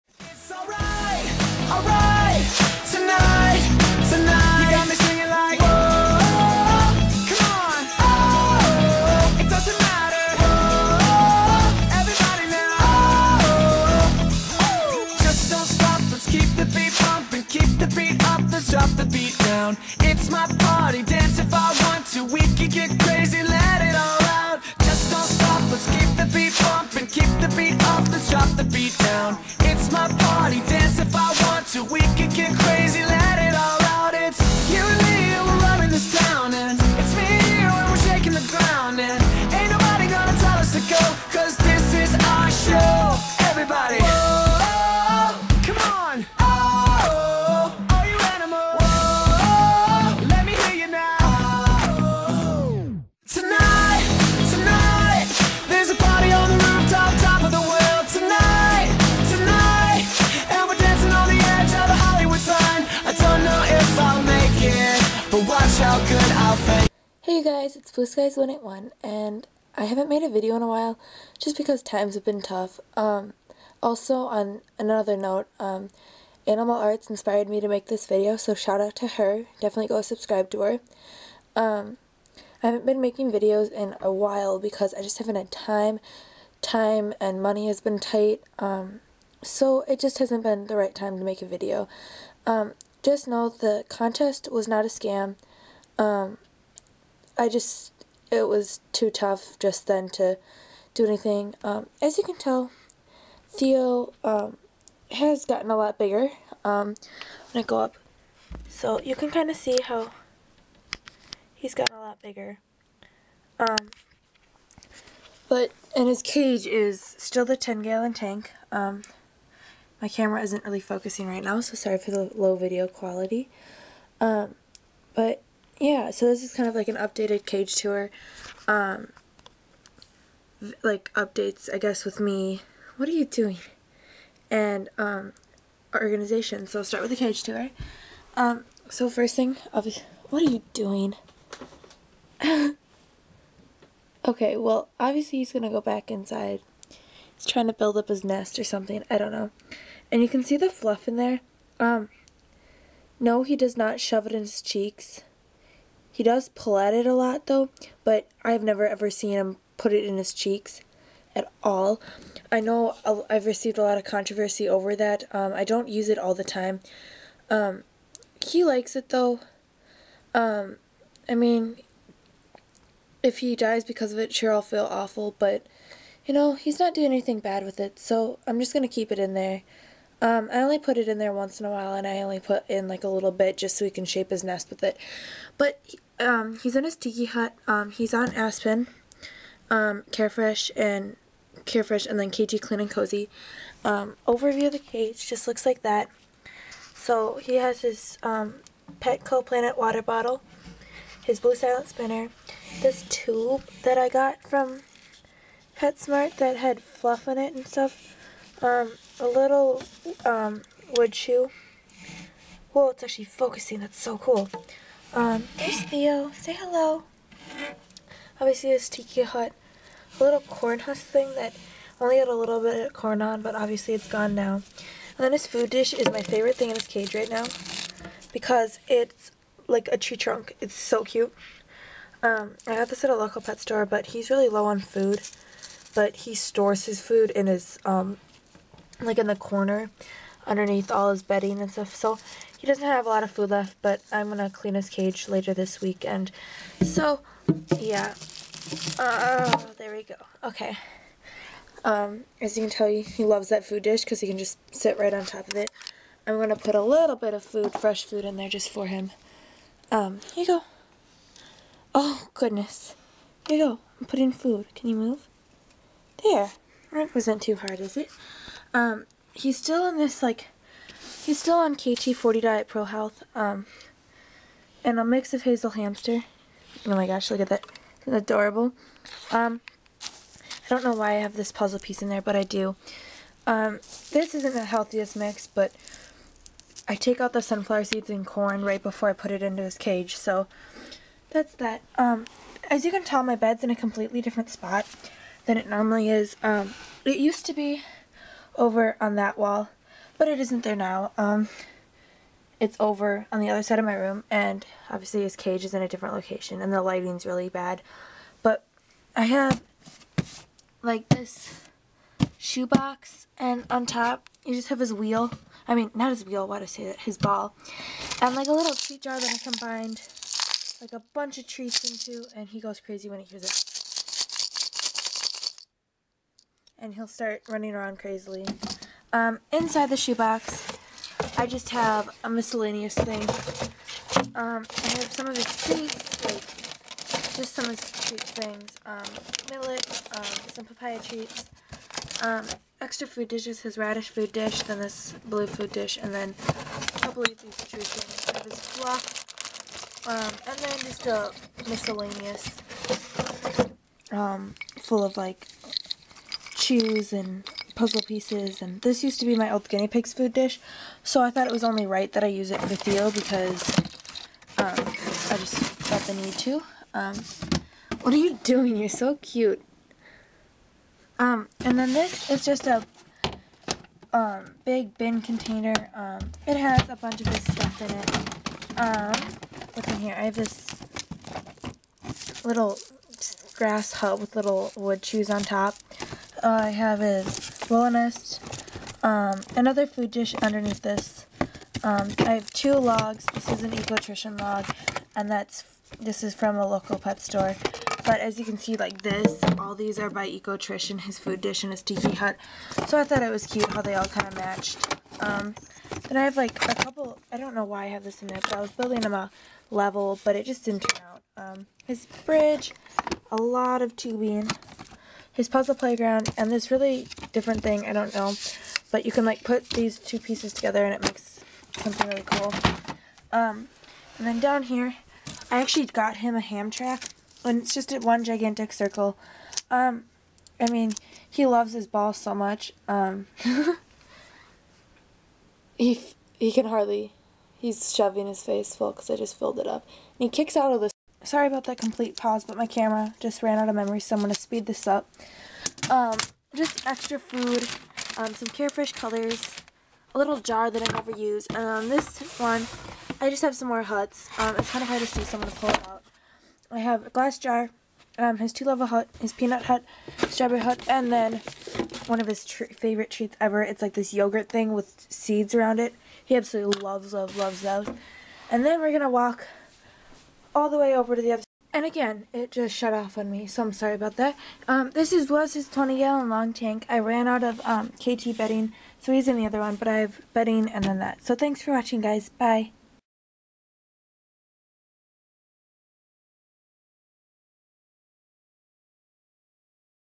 Must Watch!!
Please Read!!

As I said in the video I am truly sorry for not making a video for a while. I've been in a situation that was tough. But now I am back and better than ever!!
I will try to post as many video as I can now. Please request!!
Piper is good!! She turns 1 on may first! Time fly's doesn't it!

A very Special Thanks to AnimalArtz for inspiring me to come back! Along with many other you goys!
Thanks so much luv u  guys!